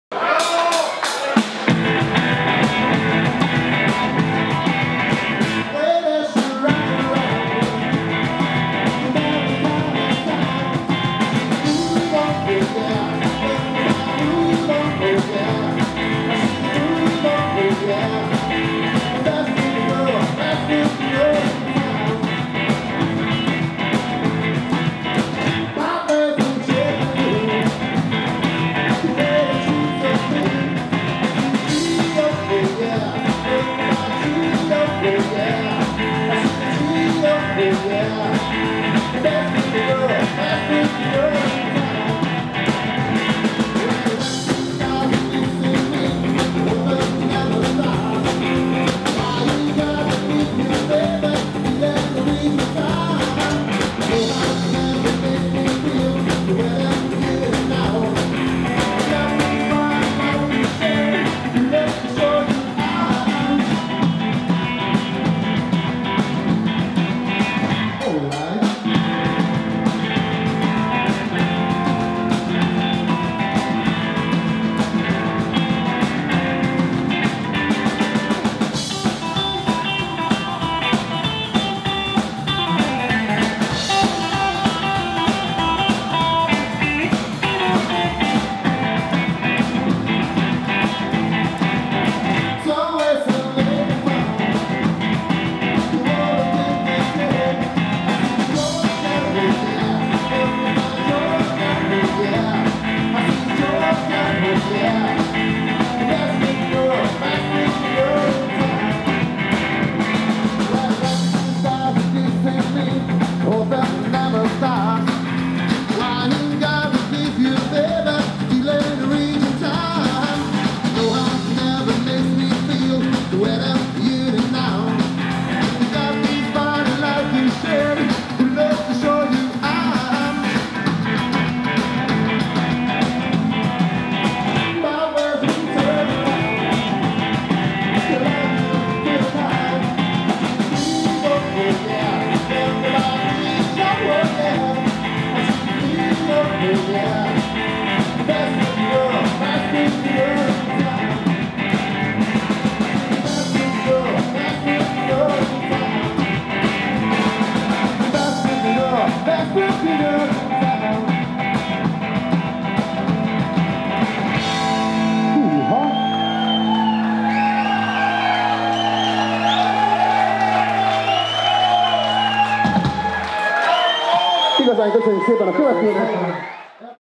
B15 - LakeSide Wekend 2016 par Tony Be Good
Le groupe B15 lors du festival LakeSide Weekend en Hongrie.